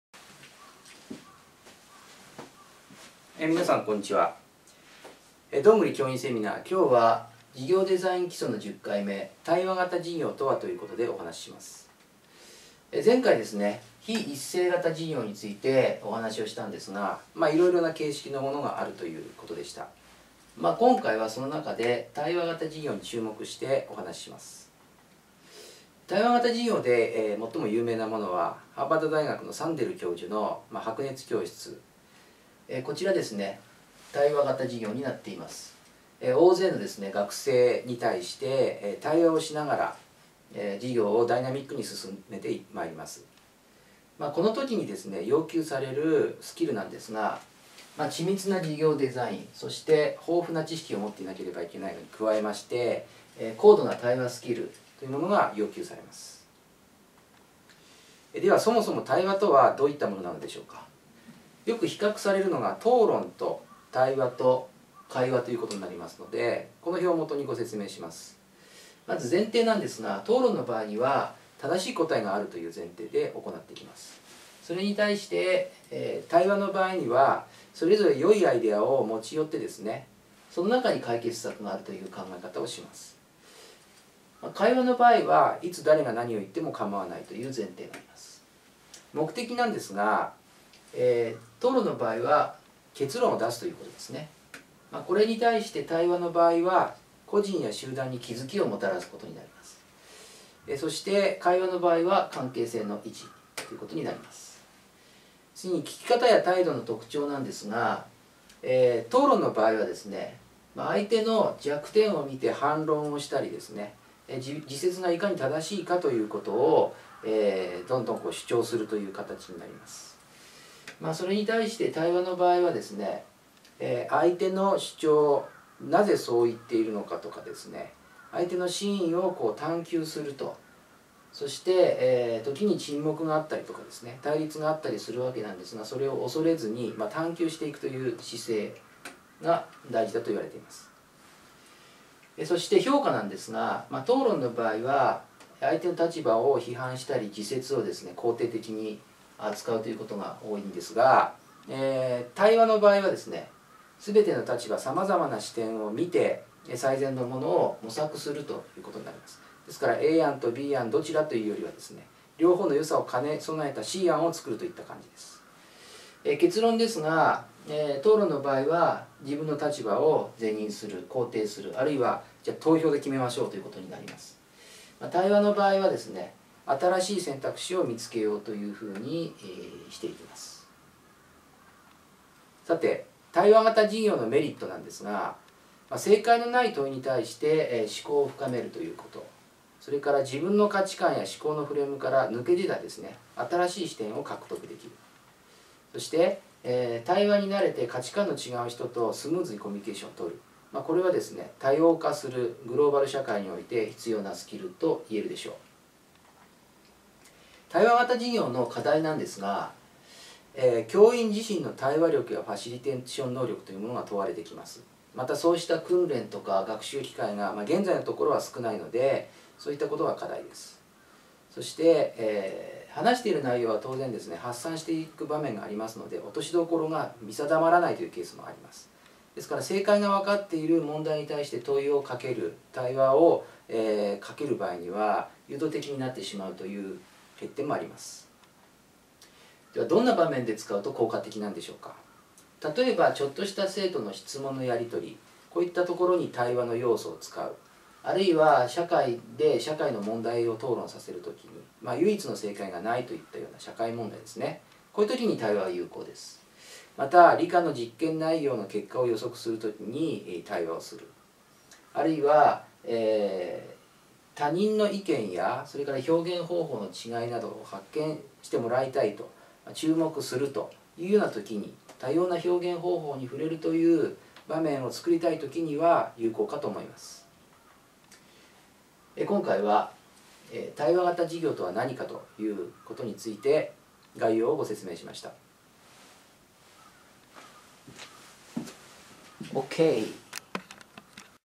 どんぐり教員セミナー017"対話型授業とは（授業デザイン基礎10）"
どんぐり教員セミナー017"授業デザインの基礎10：対話型授業とは"
非一斉型授業の一つ、対話型授業についてみていきます。
対話とは何か、対話型授業のメリットや課題、授業のこういったシーンでつかえるという提案です。

どんぐり教員セミナーについてはこちら